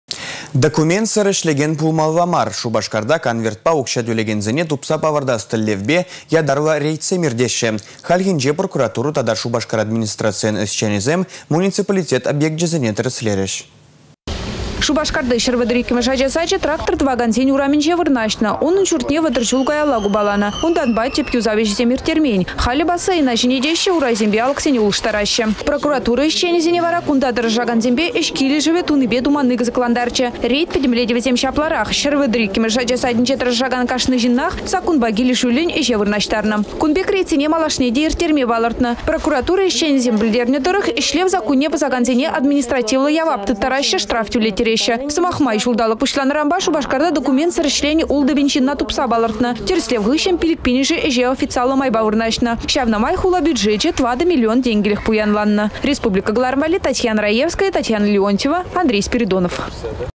Документсăр ĕçлекенсене шыраççĕ
Документсăр ĕçлекен пулмалла мар. Шупашкарта конвертпа укçа тÿлекенсене тупса палăртас тĕллевпе ятарлă рейдсем ирттереççĕ. Хальхинче прокуратура тата Шупашкар администрацийĕн ĕçченĕсем муниципалитет объекчĕсене тĕрĕслерĕç.